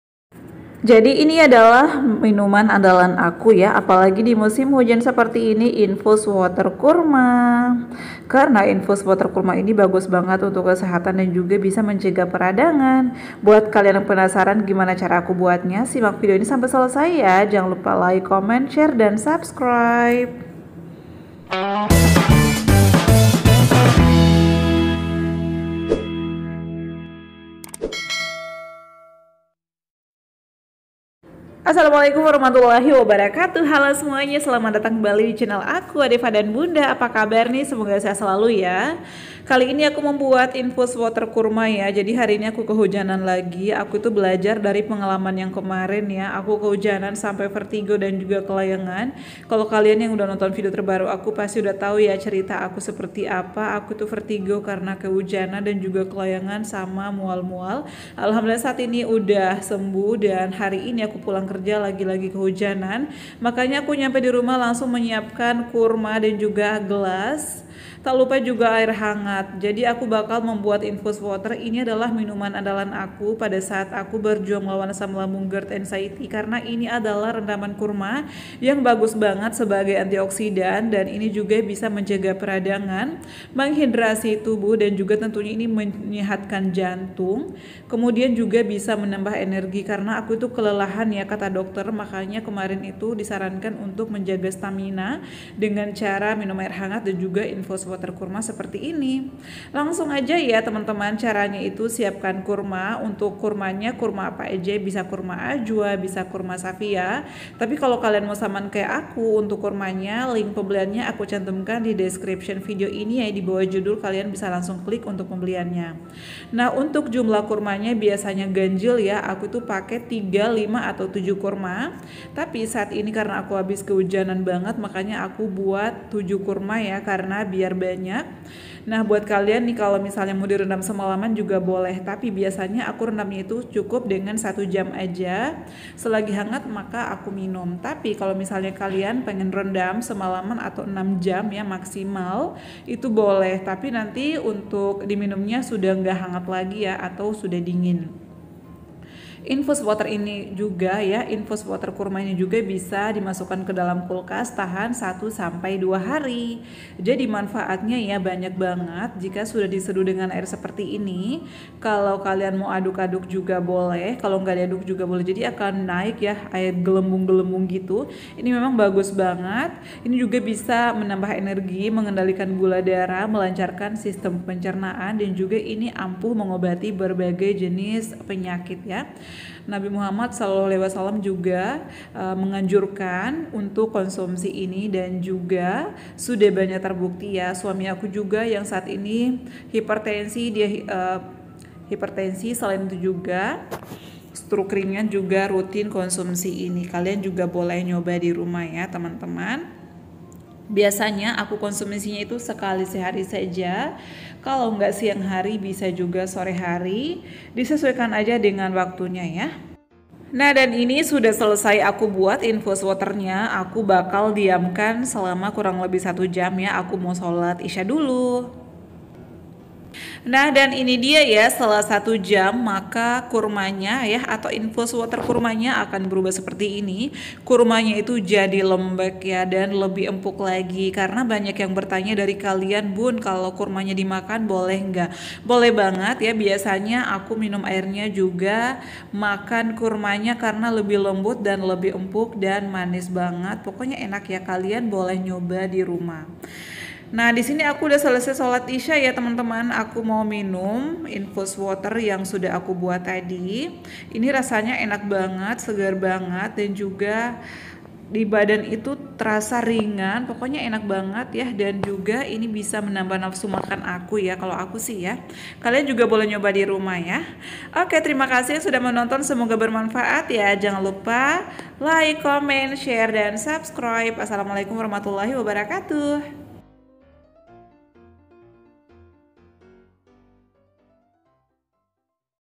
MINUMAN WAJIB AKU DIMUSIM HUJAN MENYEHATKAN LAMBUNG, MENGATASI BERBAGAI PENYAKIT - INFUSWATER KURMA
Assalamualaikum, hallo semuanya kali ini aku share cara membuat infuswater kurma minuman yang rutin aku konsumsi selama ini semoga bermanfaat 🙏